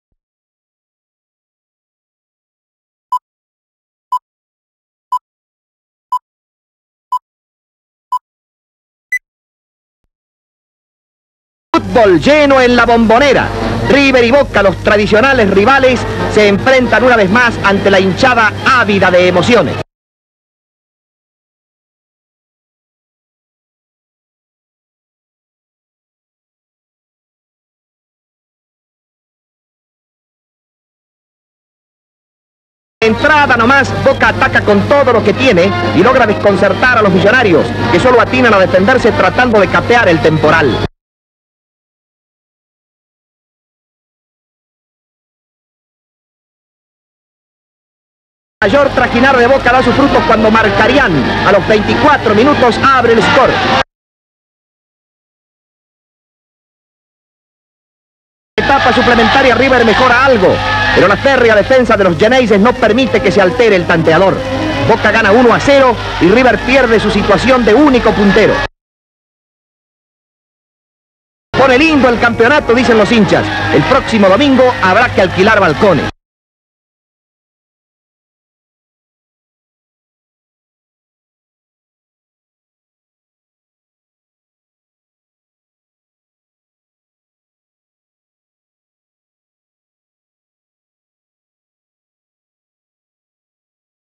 Boca Juniors vs River Plate - Primera Division 1953
Estadio La Bombonera: Boca Juniors vs River Plate (1-0), con gol de Julio Marcarian. Imágenes de los jugadores posando para la foto. Secuencias breves del partido. Vistas generales del público en las tribunas.
(Fecha 27 del torneo de Primera A)
Arbitro: John Meade
Formacion de Boca Juniors: Julio Elias Musimessi (arquero), Juan Carlos Colman, Federico Roberto Edwards, Francisco Lombardo, Eliseo Victor Mouriño, Natalio Agustín Pescia, Herminio Antonio Gonzalez, Elio Ruben Montaño, Ruben Evaristo Fernandez Real, Marcos Ricardo Busico, Julio Marcarian y Gyory Orth (D. T.).
Formacion de River Plate: Amadeo Raul Carrizo (arquero), Alfredo Ricardo Perez, Bernardo Carlos Guastavino, Roberto Tesouro, Julio Luis Venini, Pascasio Gilberto Sola, Santiago Vernazza, Eliseo R. Prado, Alberto Evaristo, Angel Amadeo Labruna y Felix Loustau.
Fecha: 1/11/1953
Duración: 1 minuto 11 segundos
Código: BG-0105